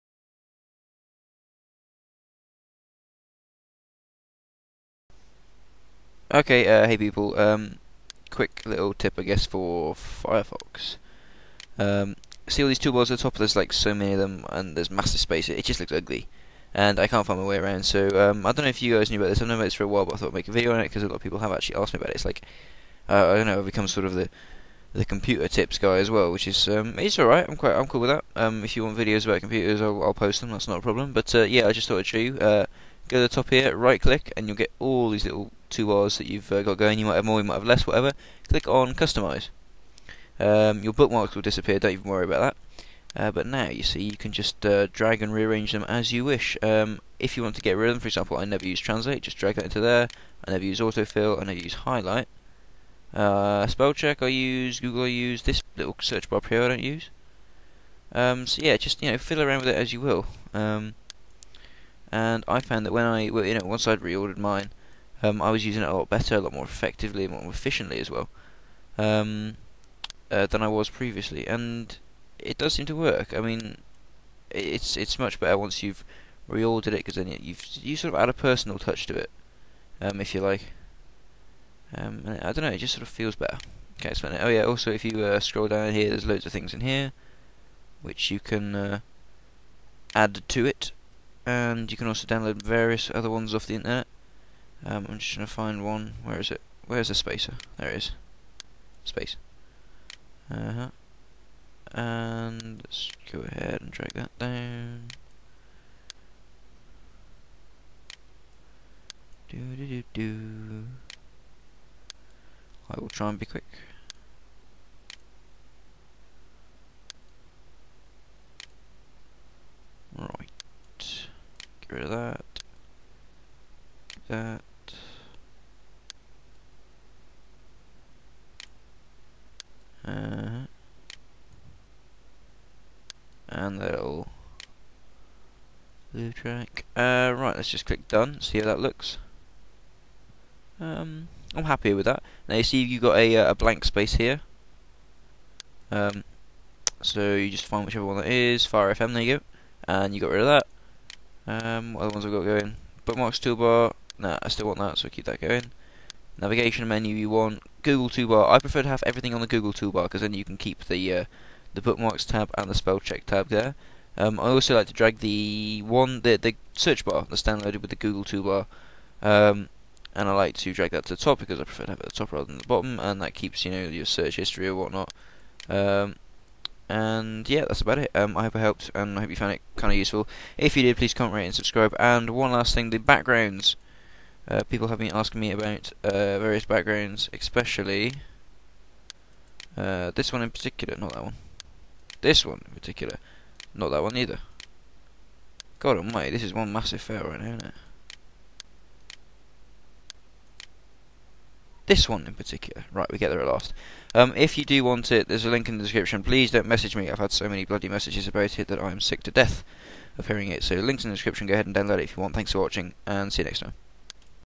Organise Your Toolbar | FireFox Tweaks
I know most of you will know this but i made a vid anywayz!!! Have an early Spring Clean of FireFox & to all those who didn't know this.